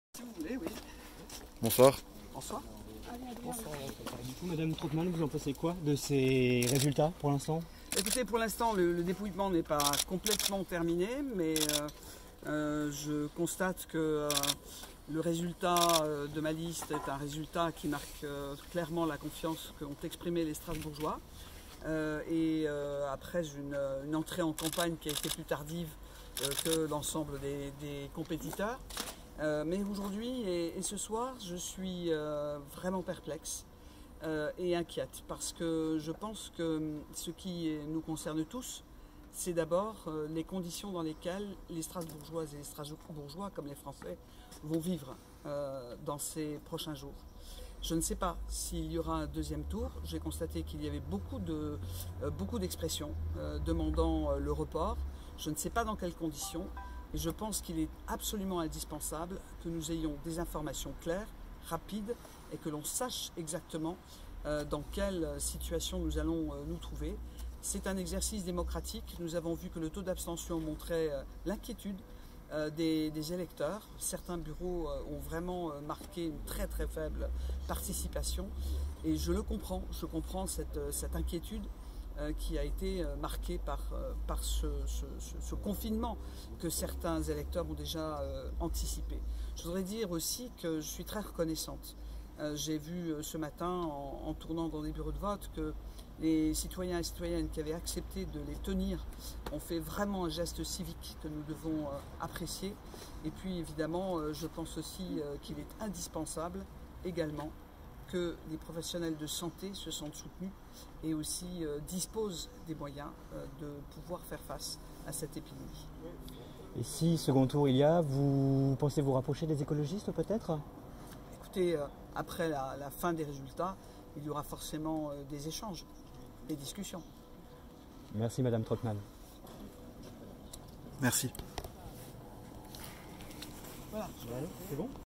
Catherine Trautmann (PS) réagit à son score au 1er tour des élections municipales
Arrivée 3e avec près de 20% des voix, Catherine Trautmann (PS) réagit à son score au 1er tour du scrutin des élections municipales à Strasbourg.